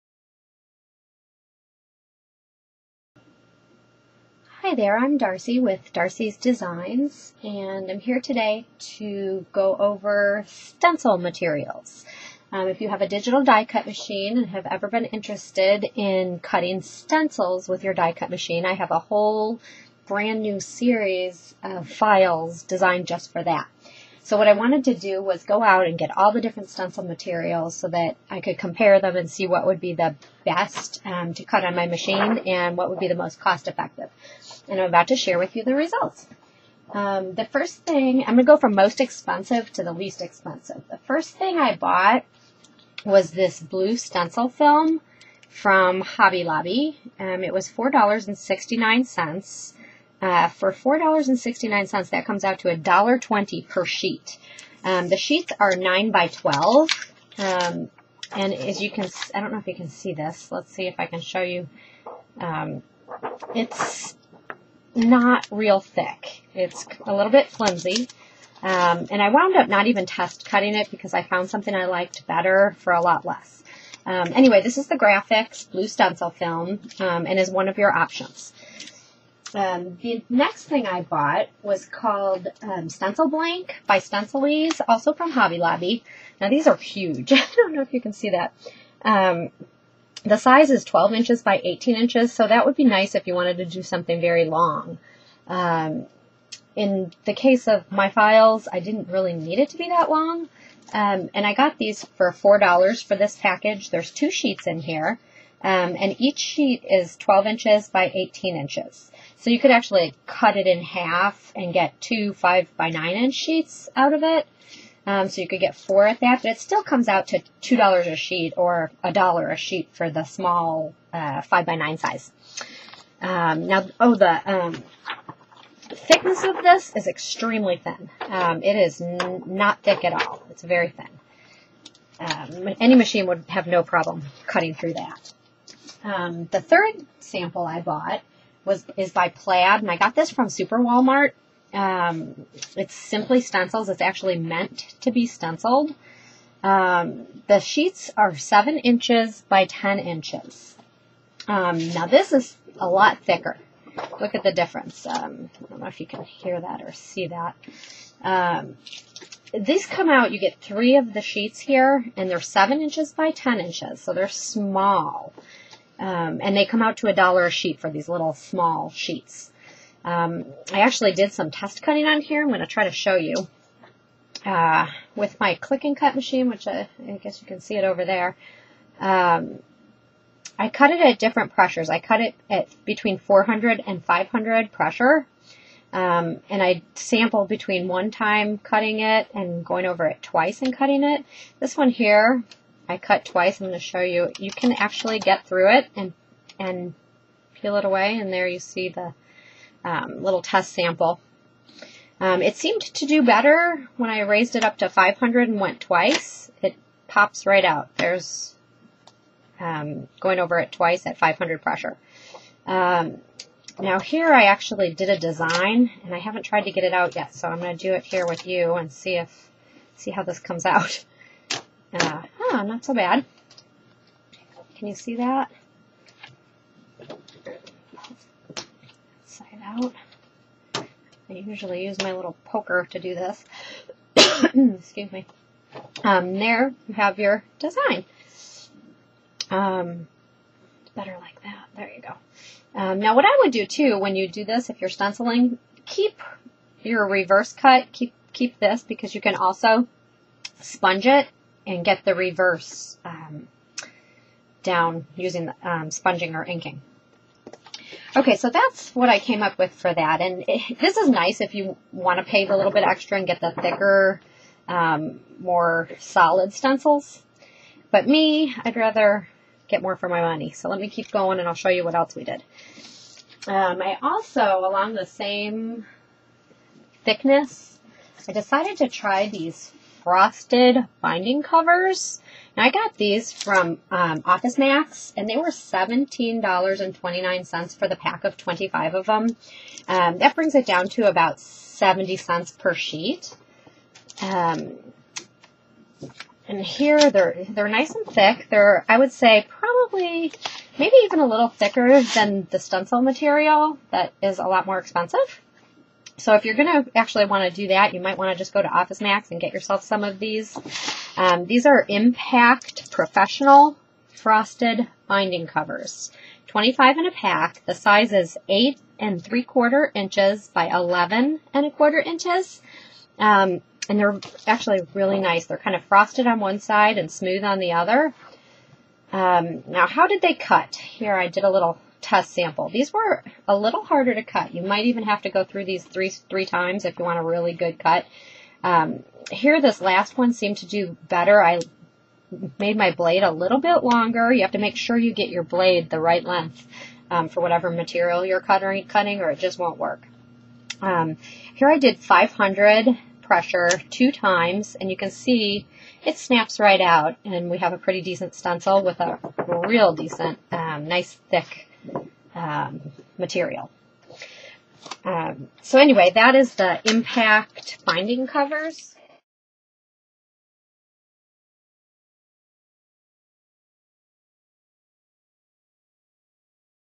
Stencil Materials & Digital Die Cutting Machines PART One
Covers different materials to use in making stencils with your digital die cutting machine.  I use a Klic N Kut to cut my stencils, but this will work in a Silhouette, Cricut, Wishblade, Gazelle, Pazzles, Craft Robo and others.